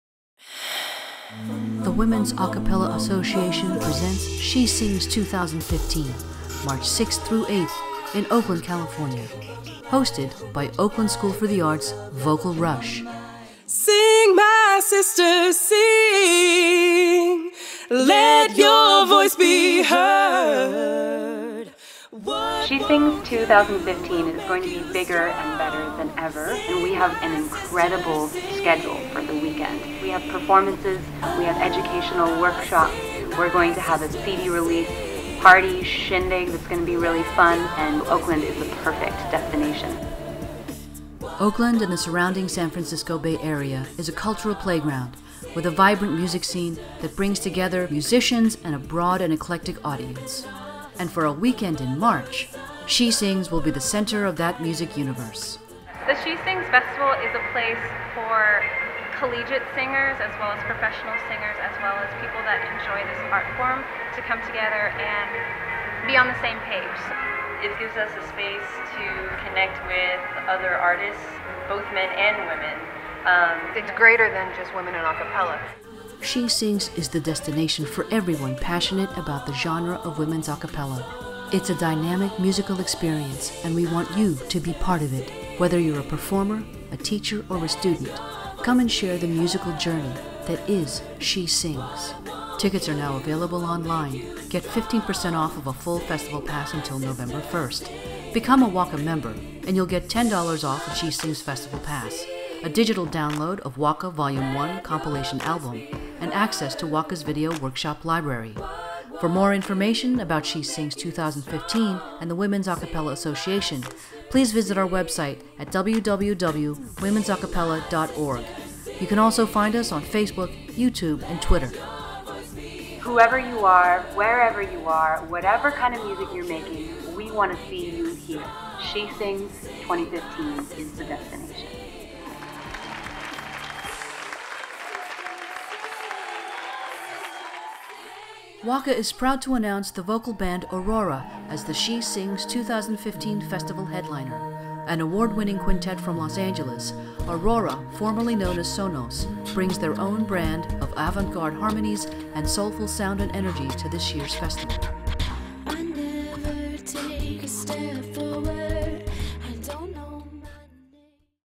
SheSings '15 Promo Video
SheSings 2015 Vocal Festival, produced by the Women's A Cappella Association, featuring ARORA!